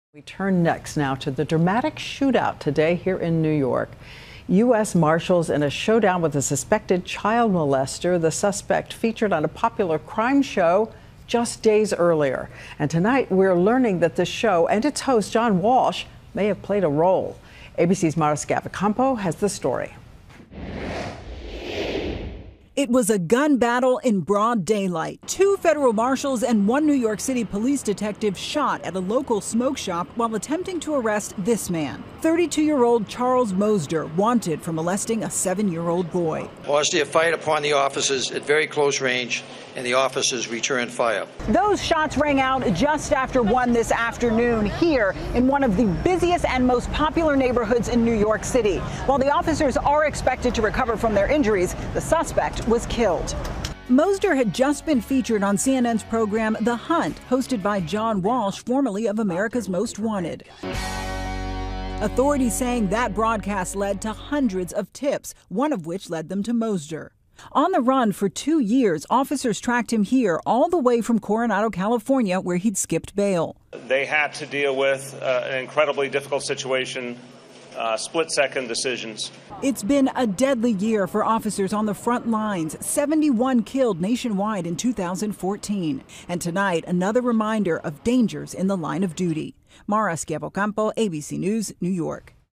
Dramatic Shootout in NY
U.S. Marshals in a showdown with suspected child molester who was featured on crime show.